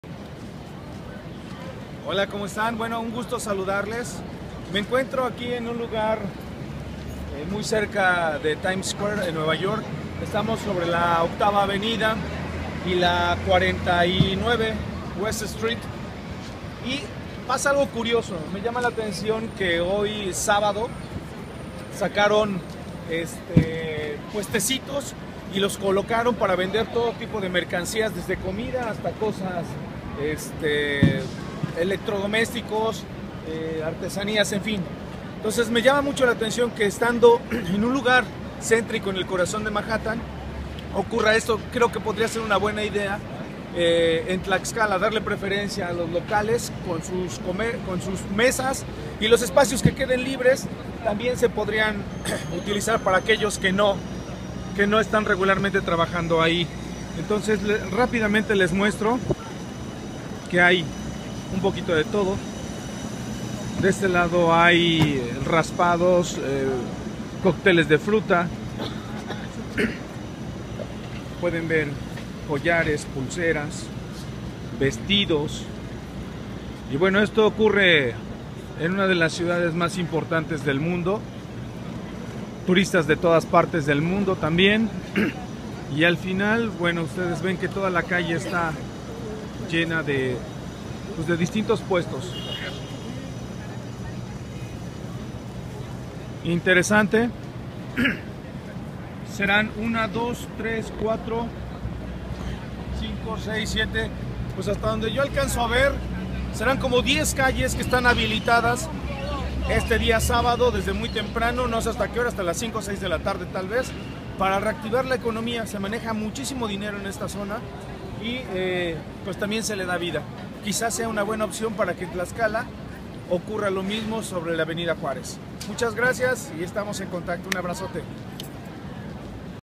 Eduardo Hernández Mercado de pulgas en Nueva York
Guardando sus proporciones y sin perder nuestra identidad, las principales avenidas de la ciudad de Tlaxcala se podrían habilitar de una manera ordenada, estética, coordinada para darle vida y dinamismo social y económico como ocurre en una de las ciudades más importantes del mundo.

Voluntad y pasión.